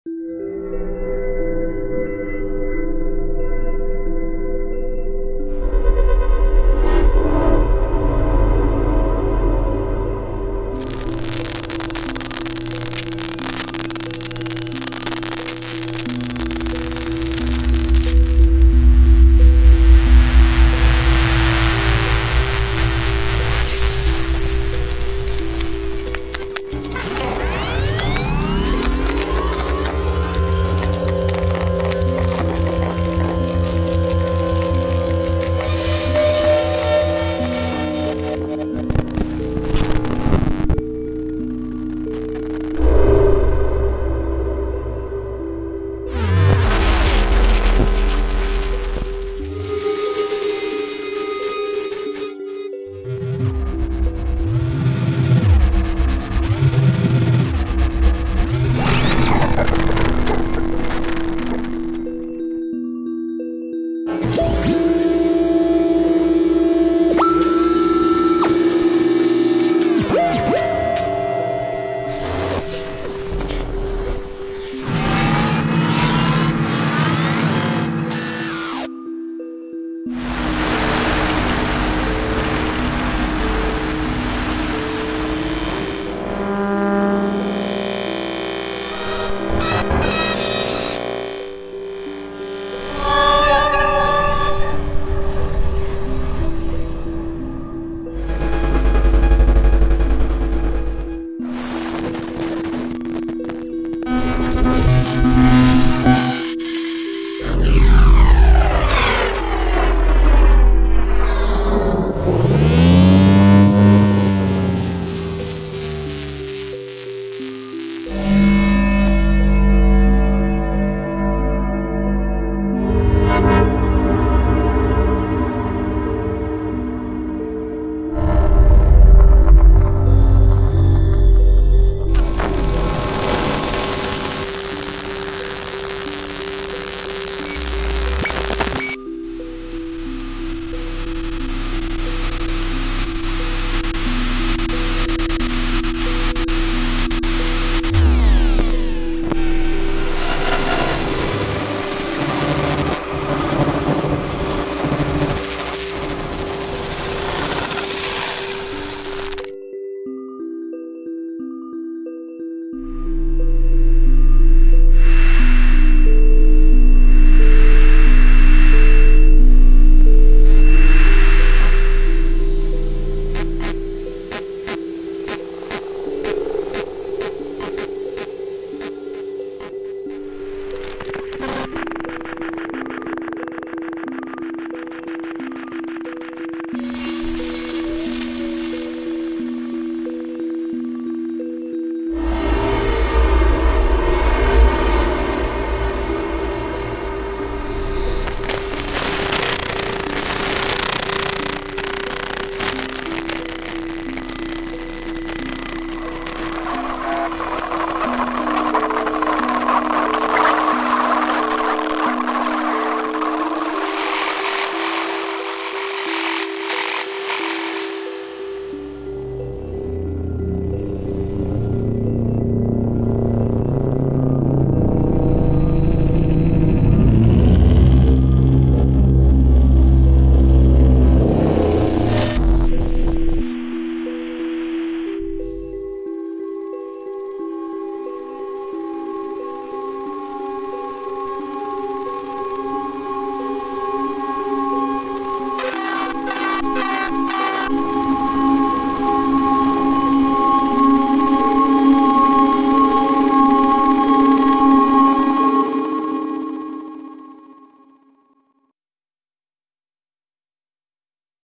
Cinematic Sci-Fi Sound Effects - Sound Design Sample Library - Designed Circuits - Trailer
Cinematic Sci-Fi Sound Effects - Designed Circuits captures the essence of electric circuits and transforms them into something new. It features special sound effects, ambiences, glitches, drones, and more, that range from heavily designed to basic elements.